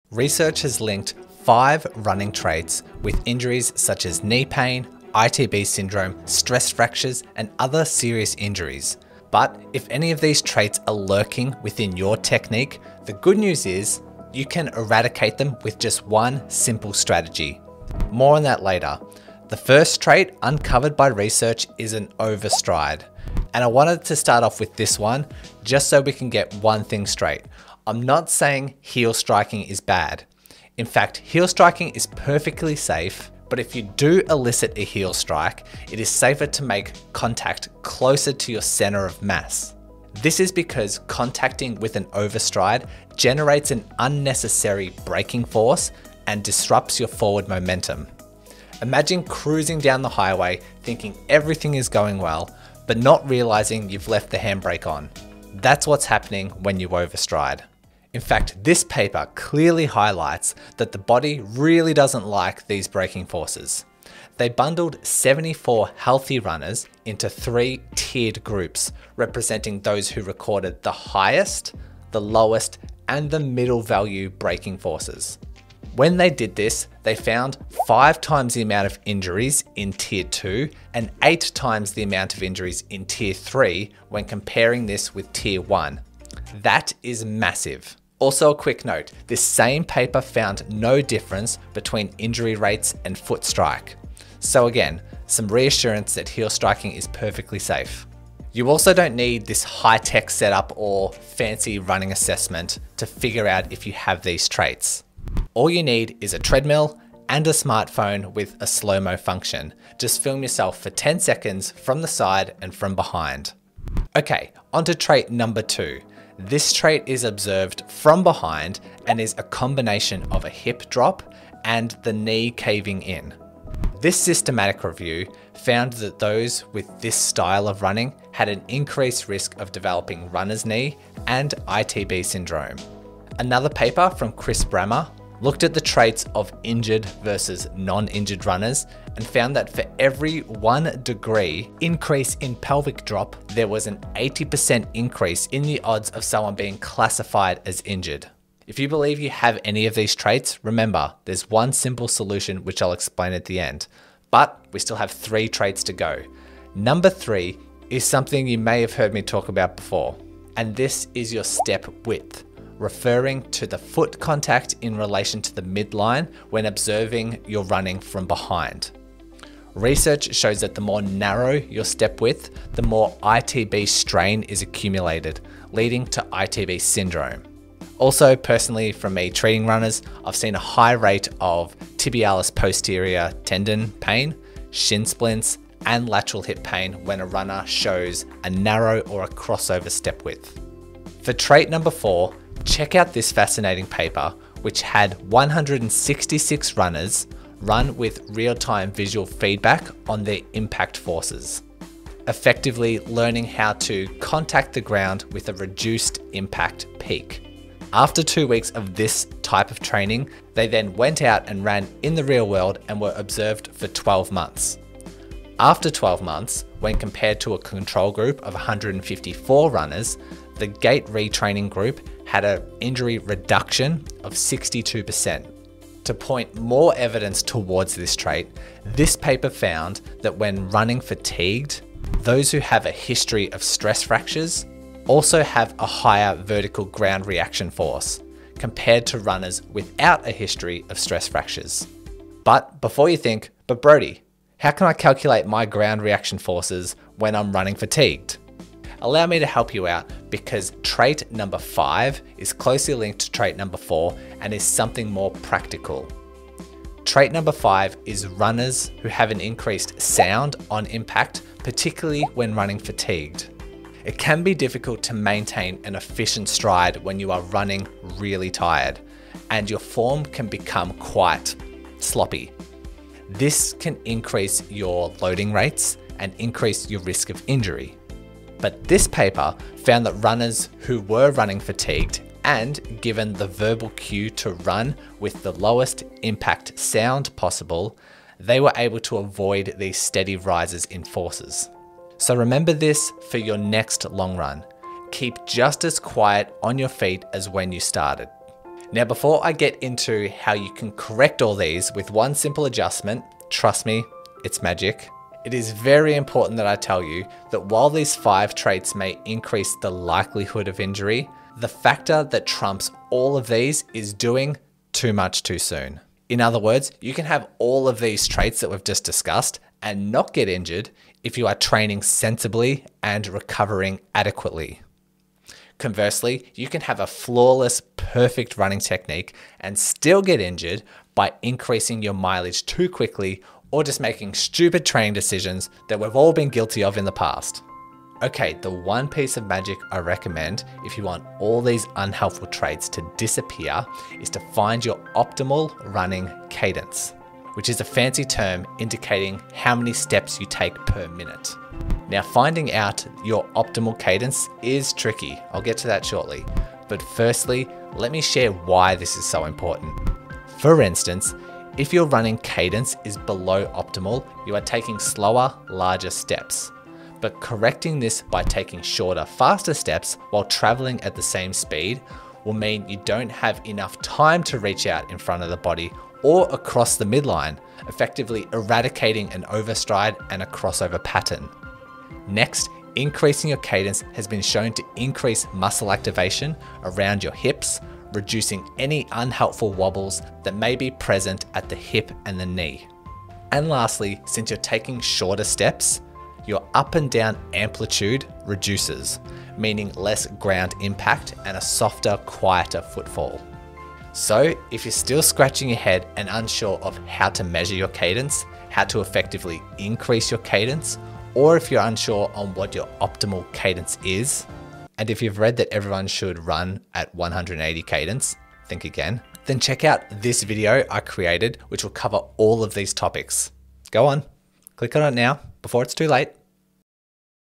Make this Simple Change to Run More Without Injury
This video dives into the fascinating research linking running technique traits with injuries such as knee pain, ITB syndrome, stress fractures, and other serious injuries.

In addition, if any of these traits are found, you will also learn how to eliminate every one of these 5 traits with just one simple strategy.

00:00 Intro
00:20 Running Trait #1: Overstriding
00:49 Breaking Forces when Running
01:00 Research with Injury & Overstride
01:40 Assessing Your Own Running
01:55 Running Trait #2: Hip Drop
02:37 Running Trait #3: Step Width
03:14 Running Trait #4: Vertical Impact Peak
03:49 Traits with Stress Fractures
04:08 Running Trait #5: Running Sound
04:39 Research with Running Quieter
05:11 The Main Reason Runners Get Injured
05:39 One Solution to Correct All Traits

Resources in this video:
Paper 1: Kinetic risk factors of running-related injuries in female recreational runners

Paper 2: Biomechanical Risk Factors Associated with Running-Related Injuries: A Systematic Review

Paper 3: Is There a Pathological Gait Associated With Common Soft Tissue Running Injuries?

Paper 5: Gait Retraining for the Reduction of Injury Occurrence in Novice Distance Runners: 1-Year Follow-up of a Randomized Controlled Trial

Paper 6: Bone strength estimates relative to vertical ground reaction force discriminates women runners with stress fracture history

Paper 7: Foot strike patterns of recreational and sub-elite runners in a long-distance road race